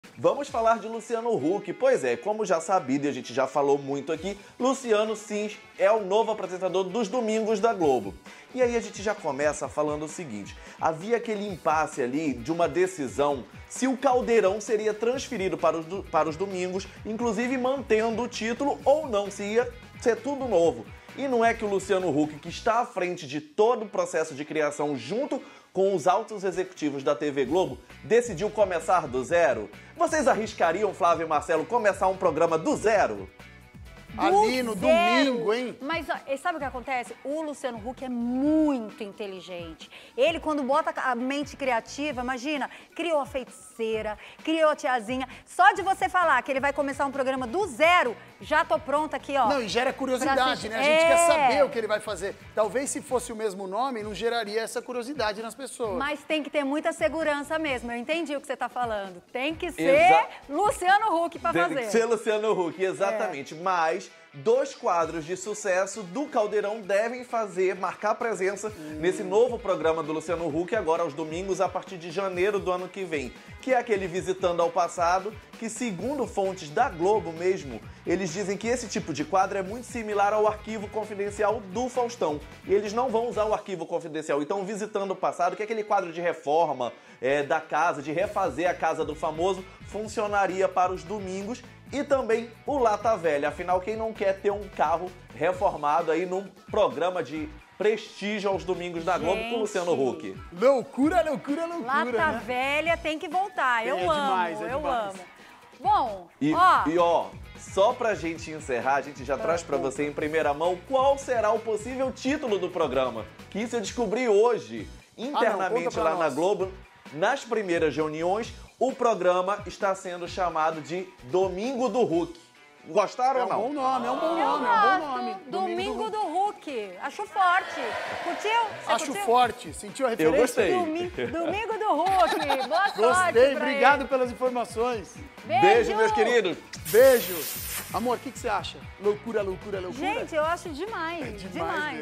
Novo programa de Luciano Huck aos domingos poderá se chamar ‘Domingo do Huck’
O repórter do TV Fama, Fa Marianno informou com exclusividade nesta sexta-feira (02) que o novo programa de Luciano Huck, que entrará ao ar no ano que vem aos domingos, poderá se chamar “Domingo do Huck”. “Isso eu descobri hoje, internamente lá na Globo”, contou o colunista